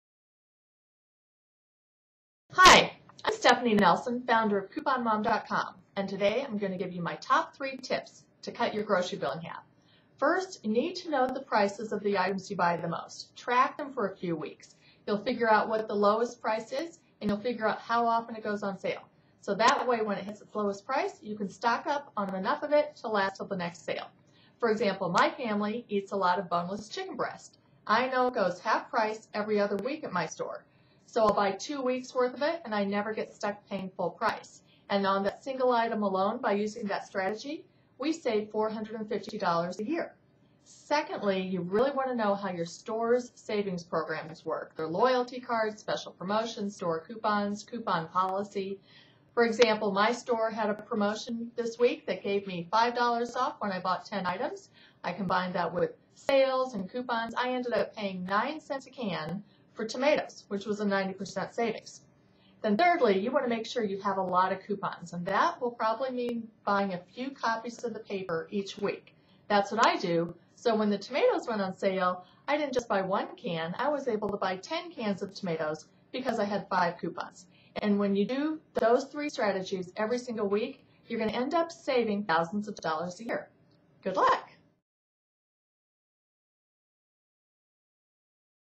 CouponMom Stephanie Nelson's Tips on How to Cut Your Grocery Bill in Half
CouponMom Stephanie Nelson (who's one of the judges in All You Magazine's America's Smartest Shopper Contest) offers up some great tips on shopping smart to save your family a lot of money.

to get more smart shopping strategies, and to enter a contest where you can win $1,000 and a photo shoot to be featured in All You Magazine!